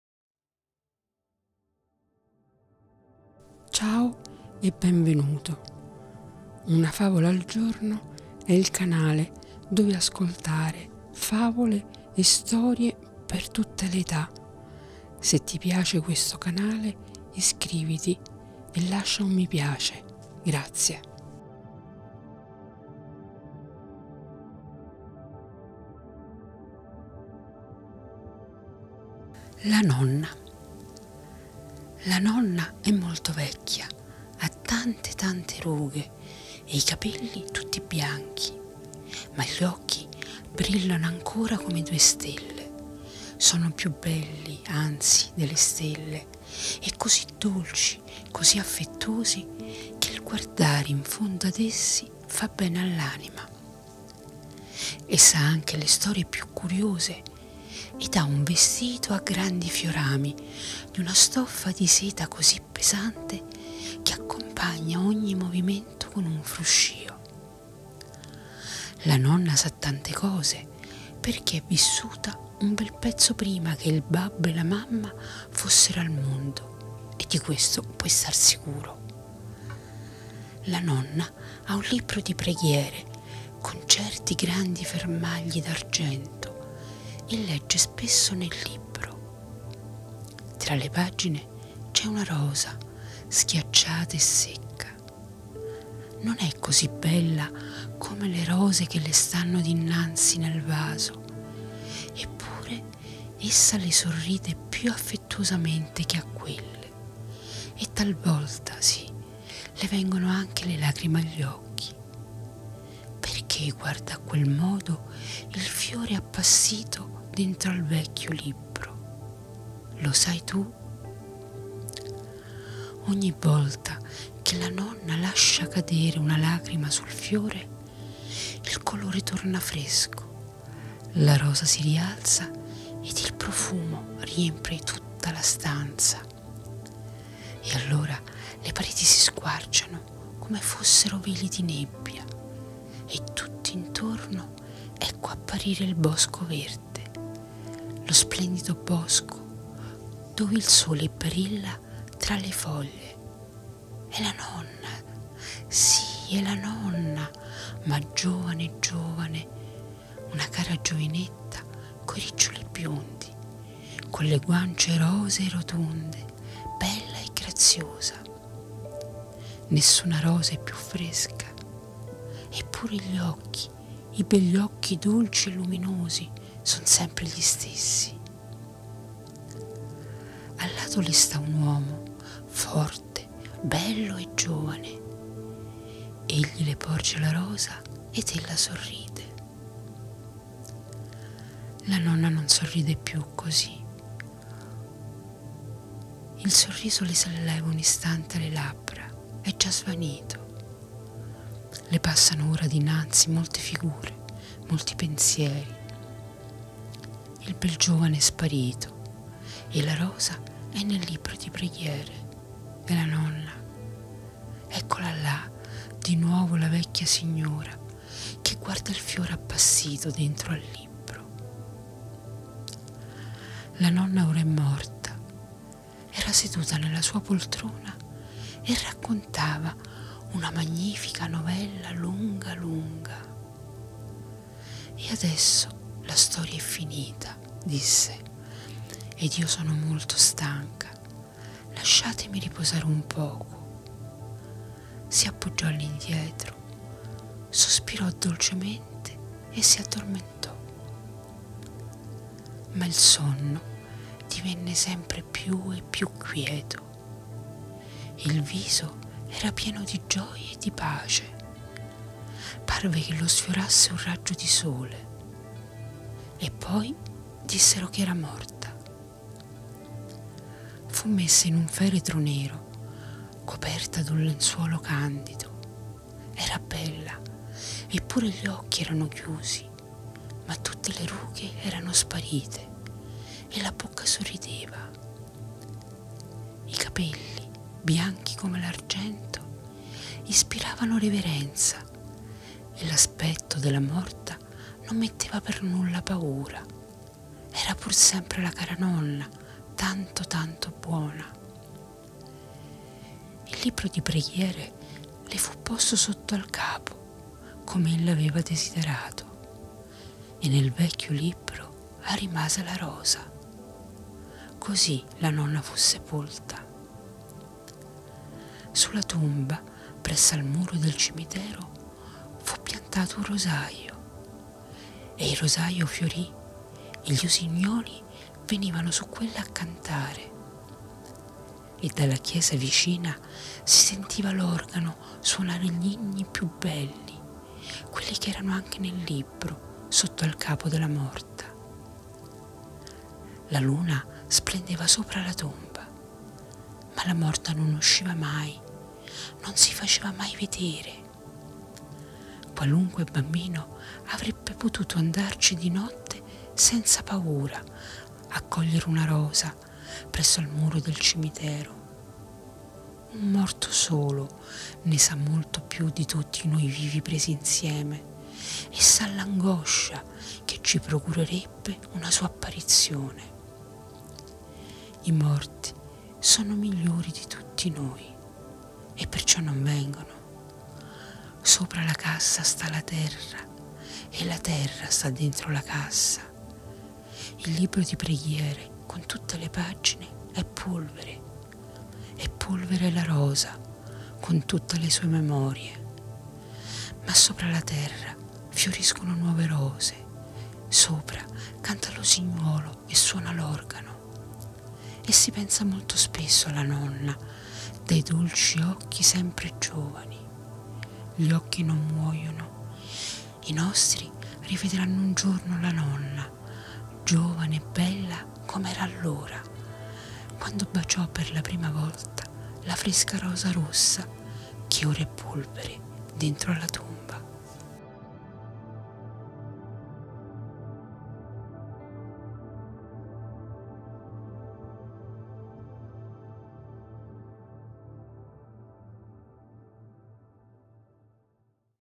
Lettura rilassante: La Nonna, una favola per bambini e adulti di Hans Christian Andersen
Racconti, Storie, Fiabe e Favole ASMR ITA
🔮 Immergiti nella dolcezza della favola "La Nonna" di Hans Christian Andersen,  perfetta per rilassarti prima di dormire o per regalarti un momento di serenità. Scopri il potere delle storie che toccano il cuore di grandi e piccini.

▬▬▬▬ LA MUSICA NEL VIDEO ▬▬▬▬
Se ti piace la musica che utilizzo nei video visita:
Relaxing Meditation Music -  Musica rilassante per meditazione